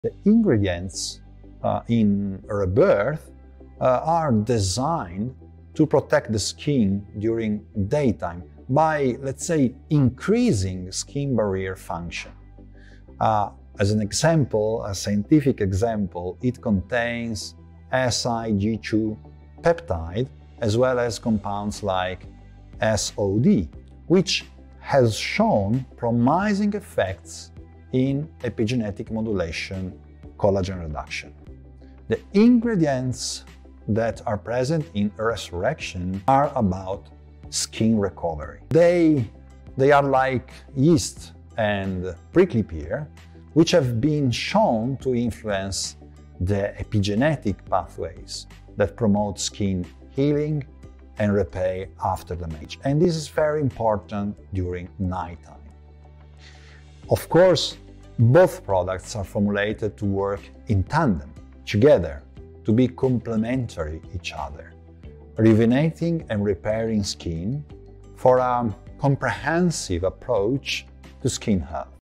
What skincare products to use? Prioritise ingredients | The expert opinion of Francesco Menegoni
⭐️ How can you choose the best skincare? By making sure your products have the best ingredients!

Rebirth and Resurrection are packed with active ingredients. They feel rich and light at the same time.

A layer of nutrition that feels like instant repair. It's literally like wearing an extra layer of skin 🧖‍♀️